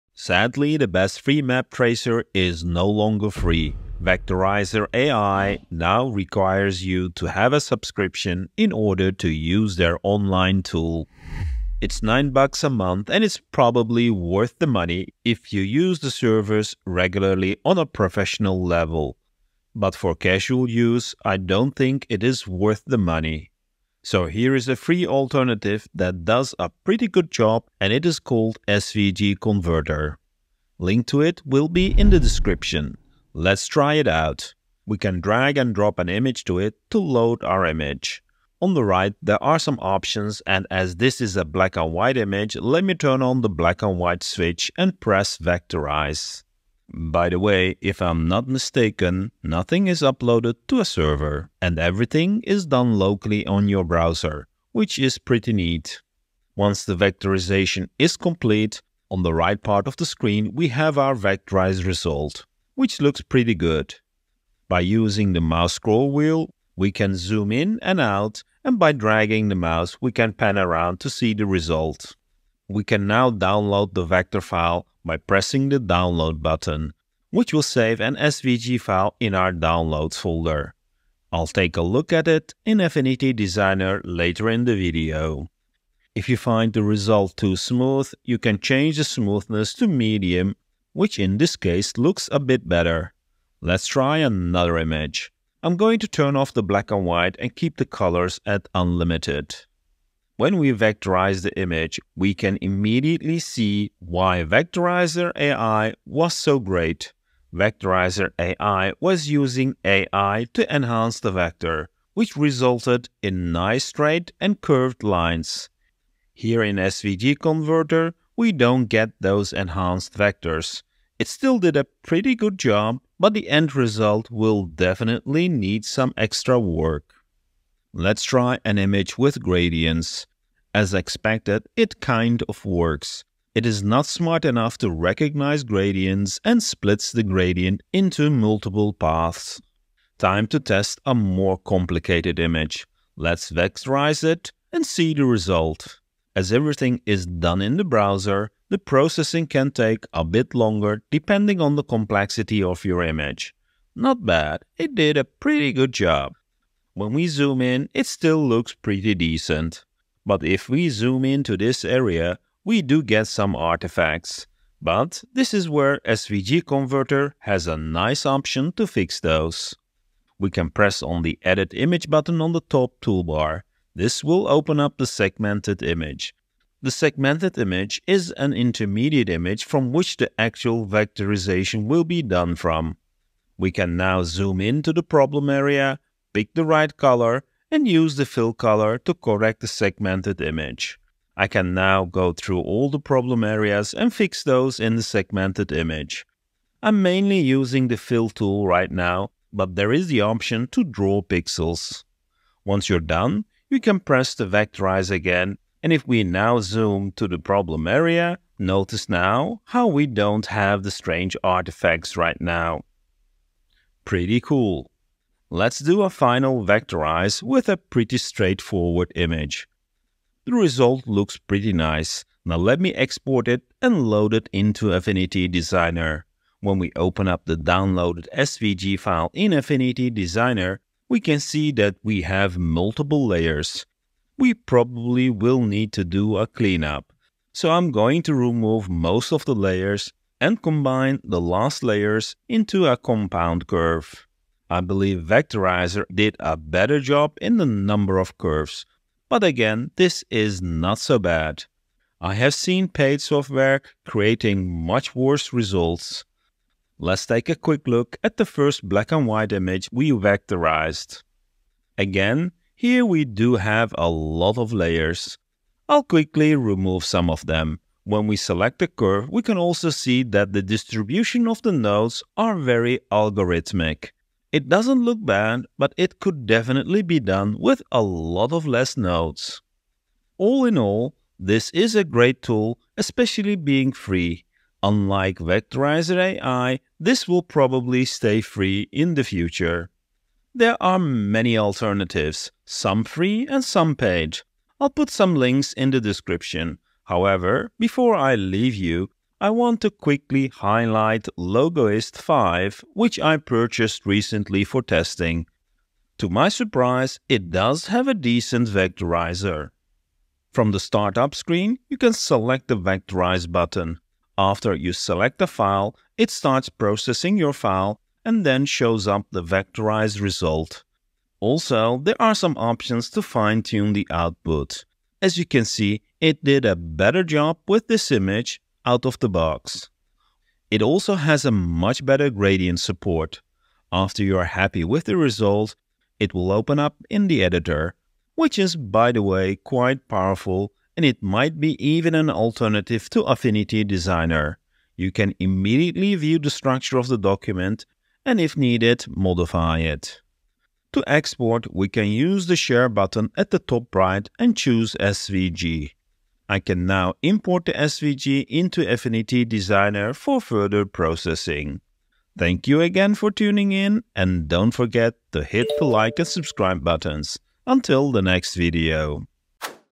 Affinity Designer 2 Trace Image 2024 - The best free tool so far
Looking for the best free option to vectorise images now vectorizer.ai is no longer free? In this video I'll share the best free tool I found so far which has by far the best results from the various free options.

We will take a quick look how it works and also take a look in Affinity Designer how the created file looks

Hope you found this video useful and thanks again for watching.